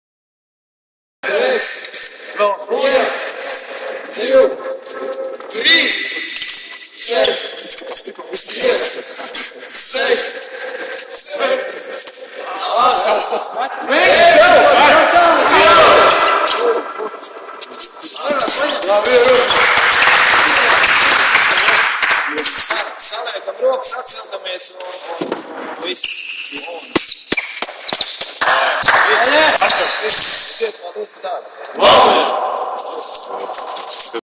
BK Valmiera komanda sveic Artjomu Butjankovu 20.dzimšanas dienā!
BK Valmiera spēlētājam Artjomam Butjankovam šodien aprit 20 gadi un visa komanda sveica Artjomu dzimšanas dienā!